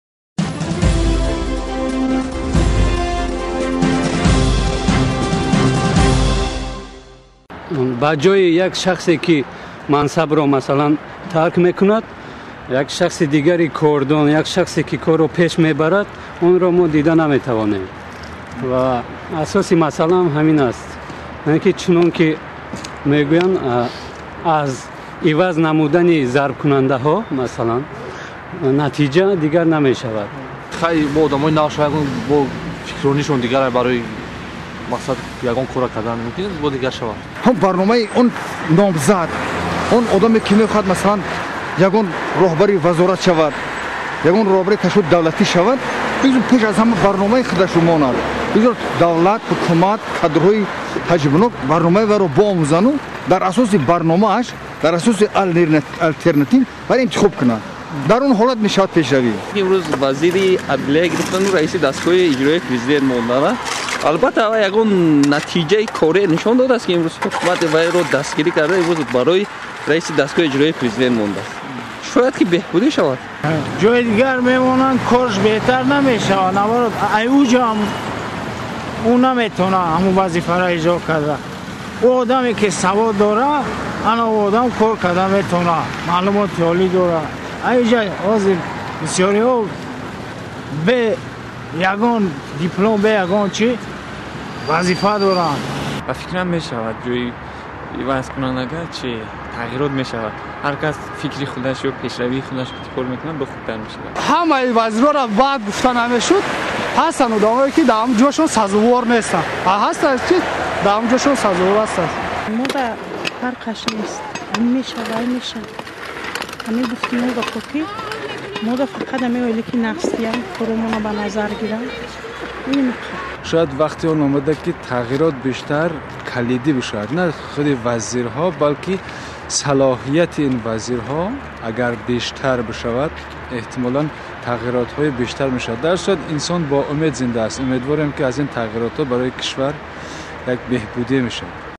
Таъсири тағйироти кадрии ҳукумат ба ҳаёти Шумо.flv
Мардуми қаторӣ хабарҳои тағйир дар ҳайати ҳукуматро баҳои гуногун медиҳанд. Бархе мегӯянд, аз ин тағйирҳо чизе дар зиндагии кишвар ва мардум иваз намешавад, вале ба ақидаи дигарон, агар шахси арзандае ба мақоме расад, ӯ метавонад, ақаллан дар ҳамон соҳа пешравие ба вуҷуд орад. Ин нукта ҳам гуфта мешавад, ки бояд на афрод, балки сиёсат тағйир ёбад.